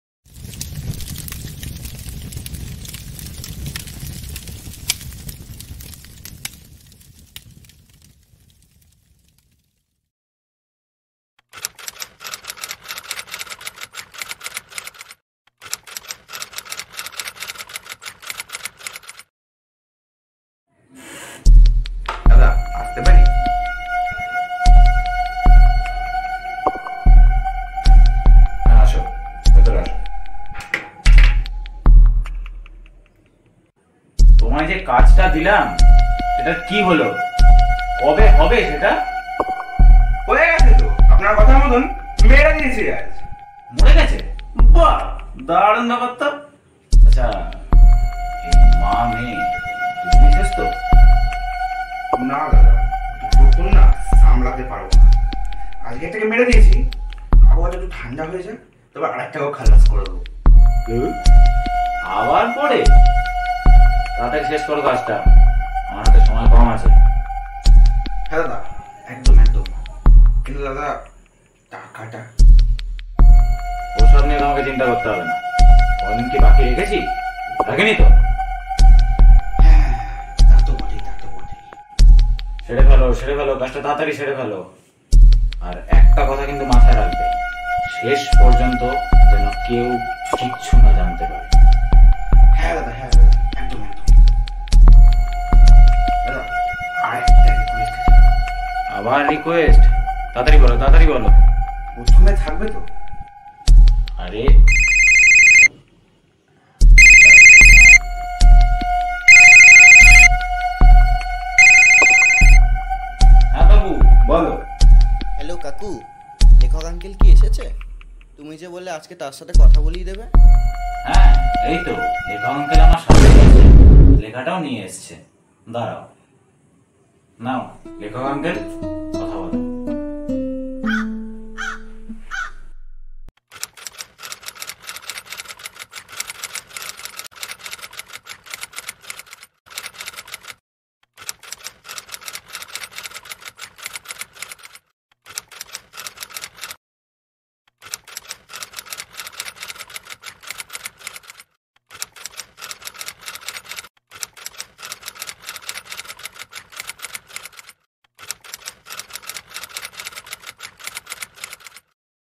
A story . directed by subhajit chakraborty #14 flamechillis house 🏘️🎬📸🎞️
act by suvam , subhajit , & devjeet.
directed by subhajit chakraborty
# flame chillis house

a funny story between a secretary & a writer